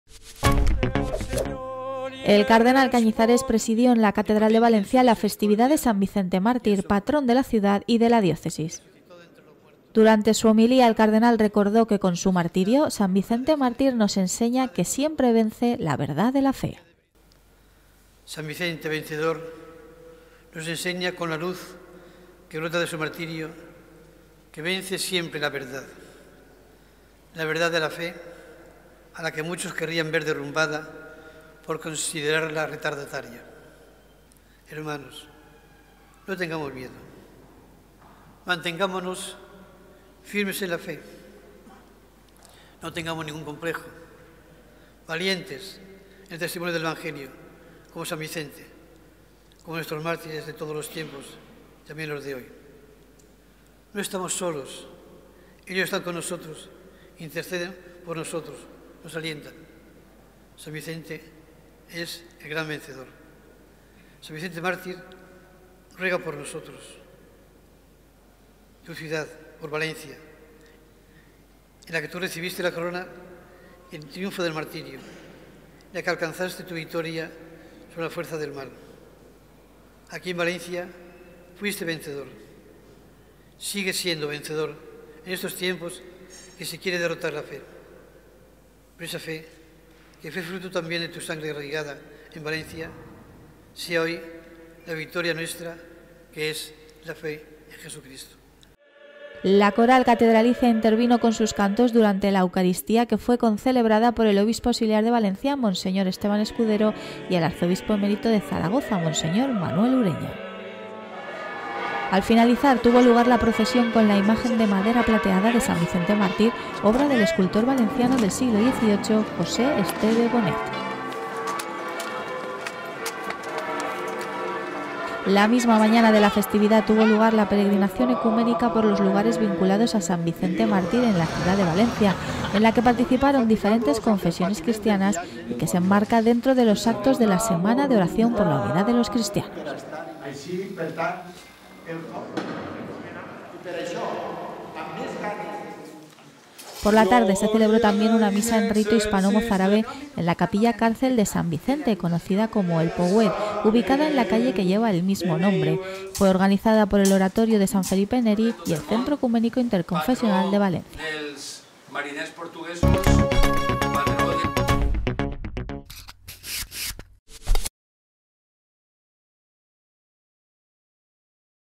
Festividad de San Vicente Mártir 2019
El cardenal arzobispo de Valencia, Antonio, Cañizares, ha asegurado hoy que “San Vicente mártir nos enseña con la luz de su martirio que siempre vence la verdad, la verdad de la fe a la que algunos querrían ver derrumbada por considerarla retardataria”.